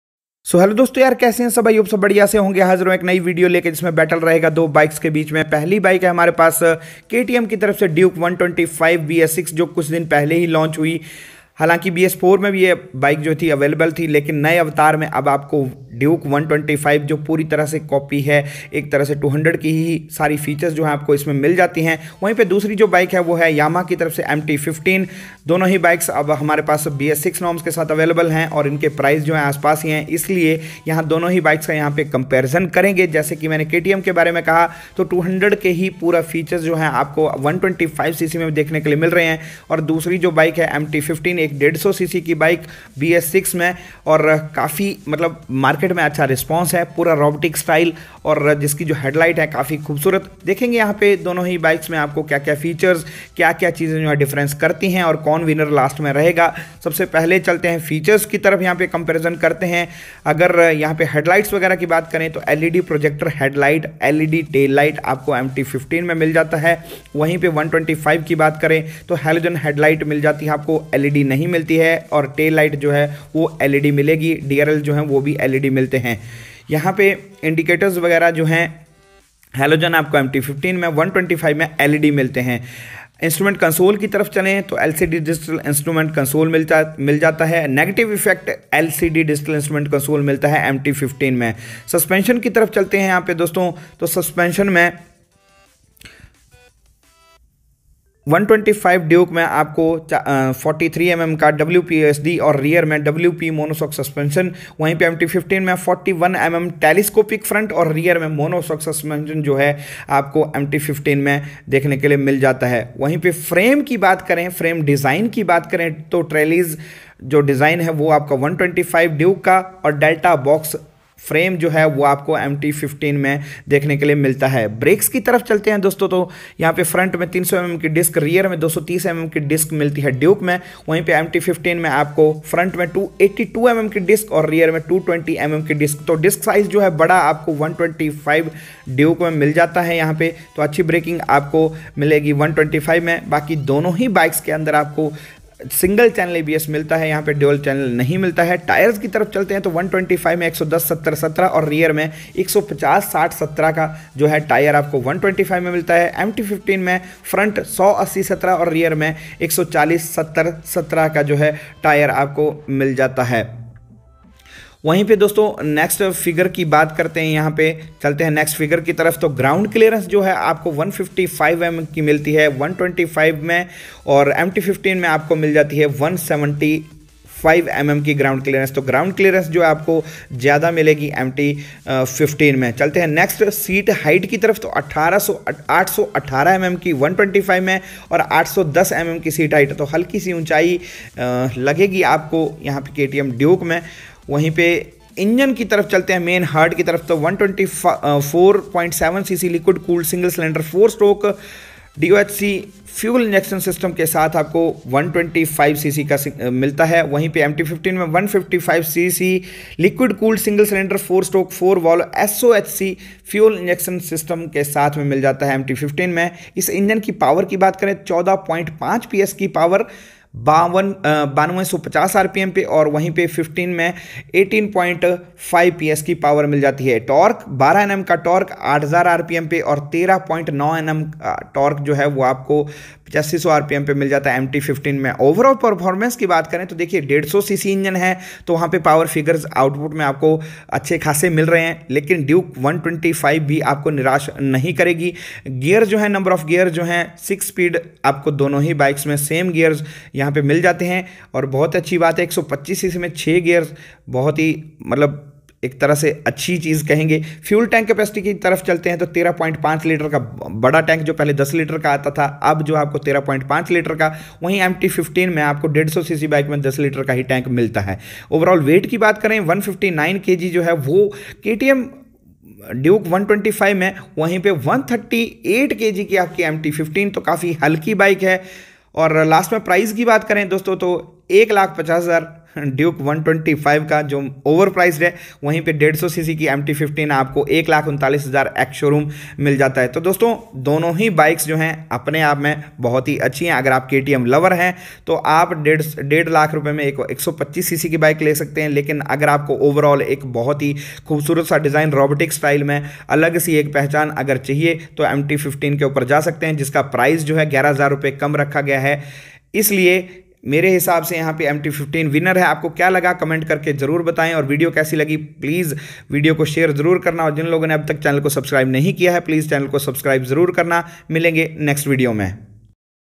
KTM DUKE 125 VS MT 15 BS6 | COMPARISON BETWEEN DUKE 125 AND MT 15 | COMPLETE DETAIL IS HERE
KTM DUKE 125 VS YAMAHA MT 15
COMPARISON BETWEEN DUKE 125 AND MT 15 COMPLETE DETAILS IS HERE